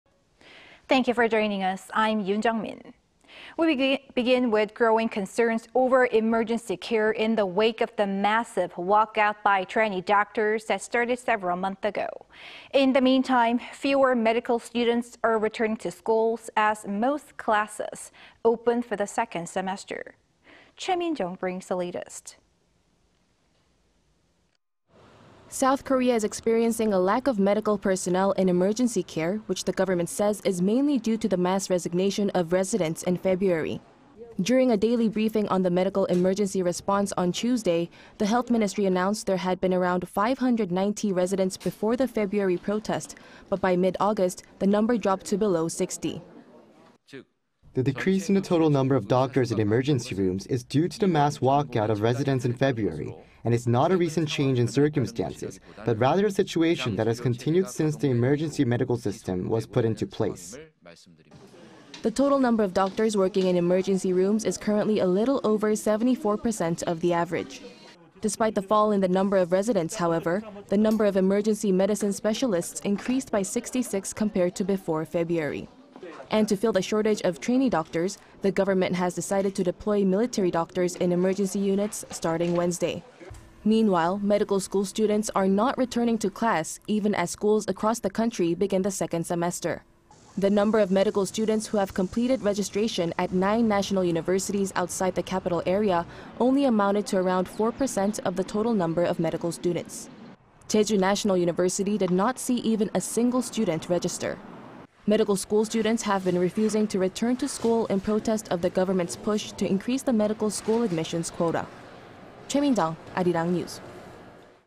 Gov't says lack of personnel in emergency care is due to resident's mass resignation
정부 "응급실 전문의 늘어… 전체 의사 감소는 전공의 이탈 때문"… 2학기 9개 국립대 복귀율 4%

Thank you for joining us. I'm Yoon Jung-min.
We begin with growing concerns over emergency care in the wake of the massive walkout by trainee doctors that started months ago.
In the meantime, fewer medical students are returning to schools as most classes opened for the second semester.
Choi Min-jung bring the latest.

South Korea is experiencing a lack of medical personnel in emergency care, which the government says is mainly due to the mass resignation of residents in February.
During a daily briefing on the medical emergency response on Tuesday,. the health ministry announced there had been around 5-hundred-90 residents before the February protest, but by mid-August, the number dropped to below 60.

"The decrease in the total number of doctors in emergency rooms is due to the mass walkout of residents in February, and is not a recent change in circumstances, but rather a situation that has continued since the emergency medical system was put into place."

The total number of doctors working in emergency rooms is currently a little over 74 percent of the average.
Despite the fall in the number of residents, however, the number of emergency medicine specialists increased by 66 compared to before February.
And to fill the shortage of trainee doctors, the government has decided to deploy military doctors in emergency units starting Wednesday.

Meanwhile, medical school students are not returning to class, even as schools across the country begin the second semester.
The number of medical students who have completed registration at nine national universities outside the capital area, only amounted to around 4-percent of the total number of medical students.
Jeju National University did not see even a single student register.
Medical school students have been refusing to return to school in protest of the government's push to increase the medical school admissions quota.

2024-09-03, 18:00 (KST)